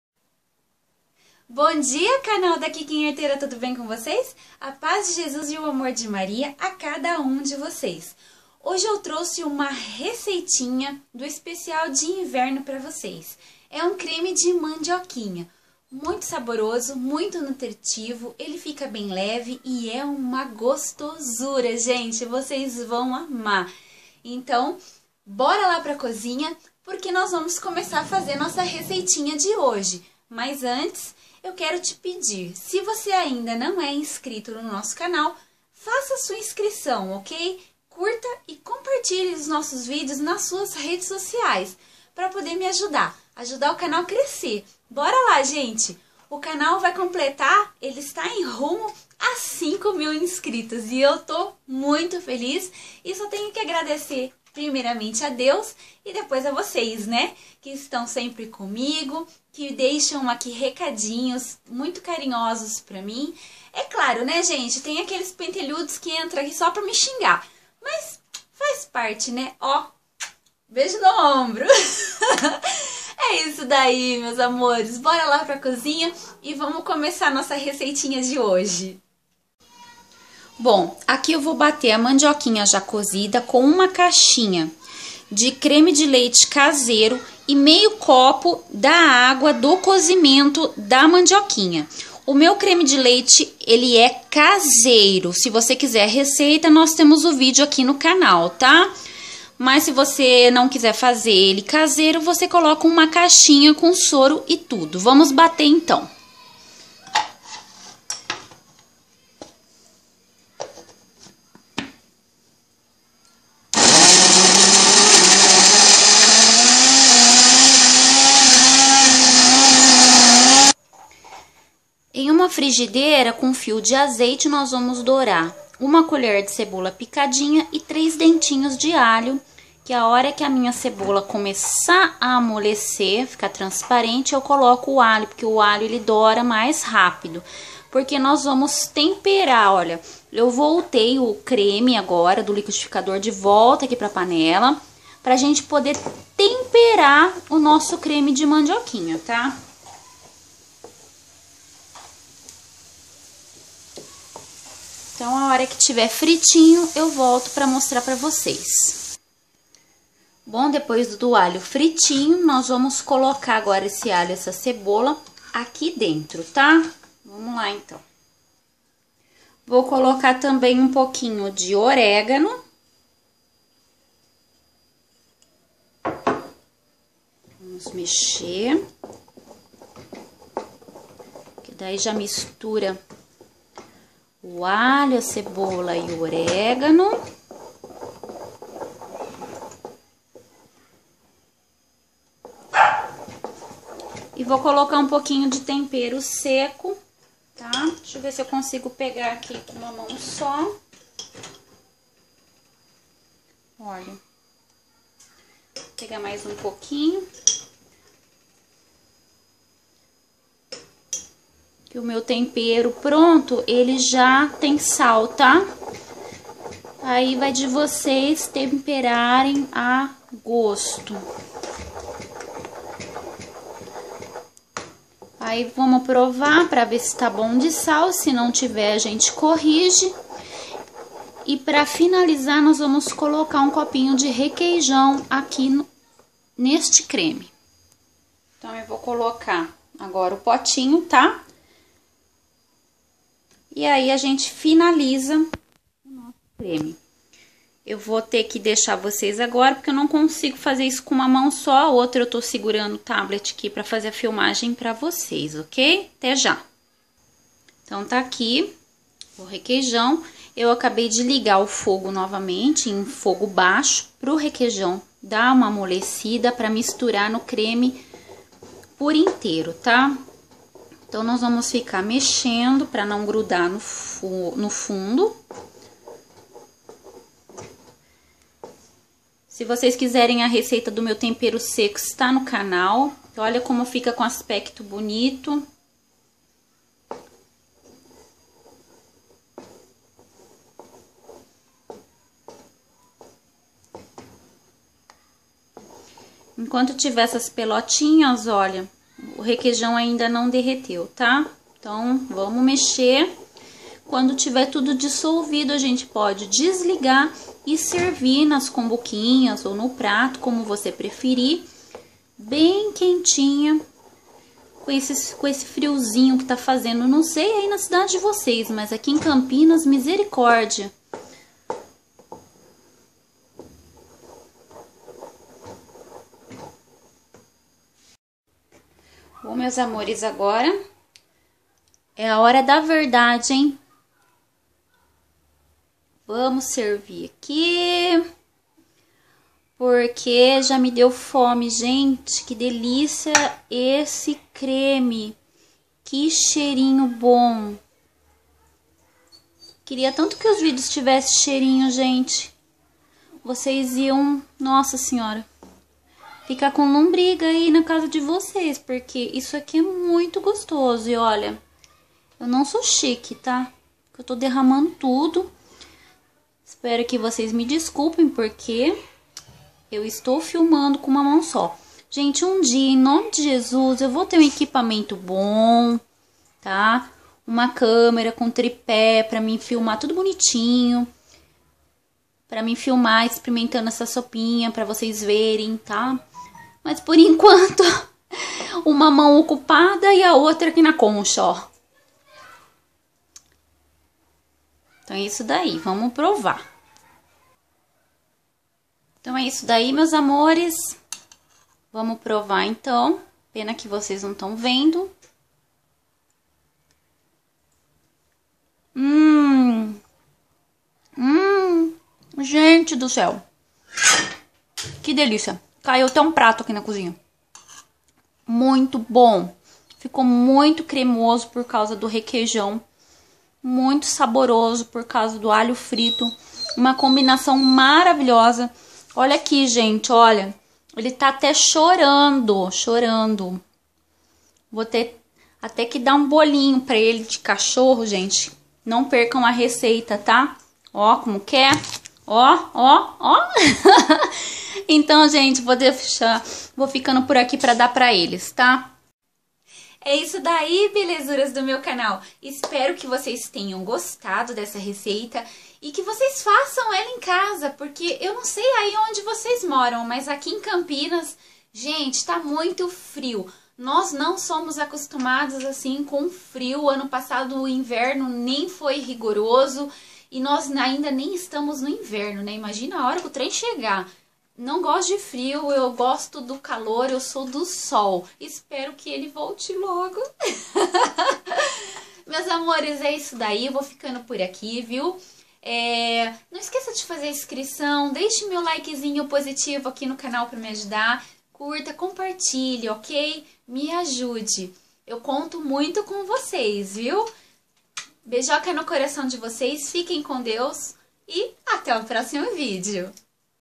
Creme de Mandioquinha com Requeijão
Quando chega o inverno, tudo que nós pensamos são em Receitas de Sopas, Caldos e Cremes para esquentar as nossas noites
Este creminho fica super cremoso e a combinação dos ingredientes é muito saborosa!

✉ Contato Kikinha Arteira​:
(Anúncios/Parcerias/Negócios)]

falando um pouquinho de mim